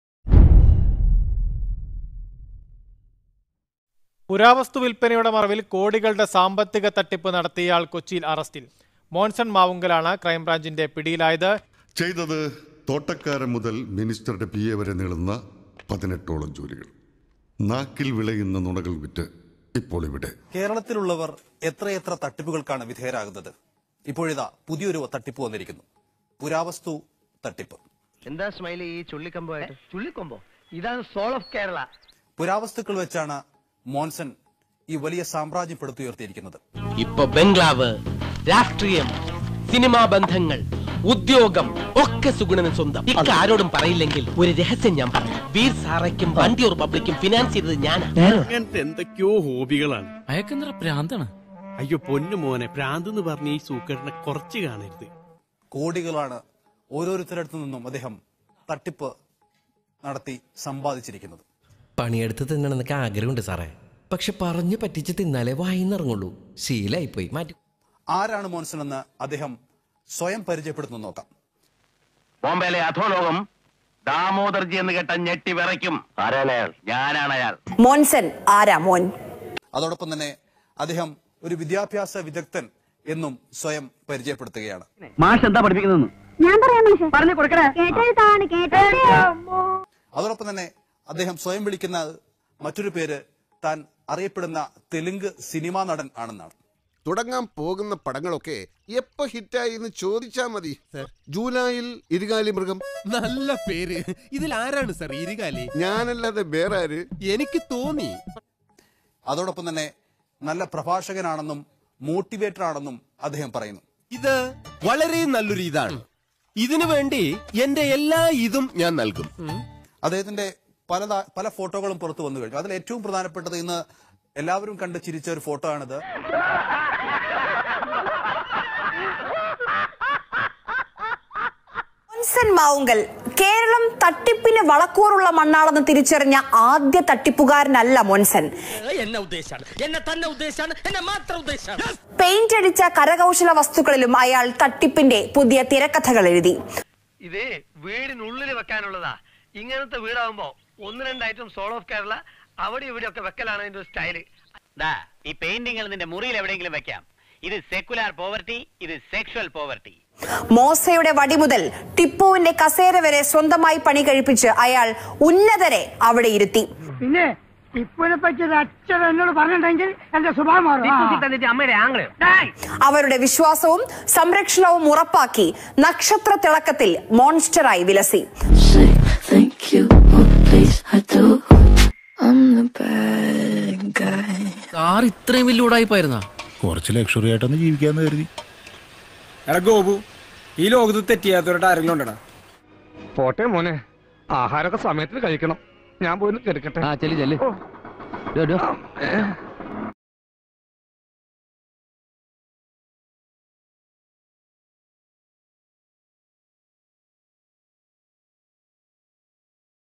സോൾ ഓഫ് കേരള പോലീസ് പൊക്കി /മോൻസൺ ട്രോൾ
monsantroll trollmalayalam ഇതിനും വലുത് എന്തോ വരാനിരുന്നതാണ് അത് ഇങ്ങനെ കഴിഞ്ഞു എന്ന് സമാധാനിക്കുക 😂.. അല്ലെങ്കിൽ പുരാവസ്തു ആണ് എന്ന് പറഞ്ഞു... വേറെ എന്തങ്കിലും കൊണ്ടുവരുമായിരുന്നു 😂👍@monsonmavunkal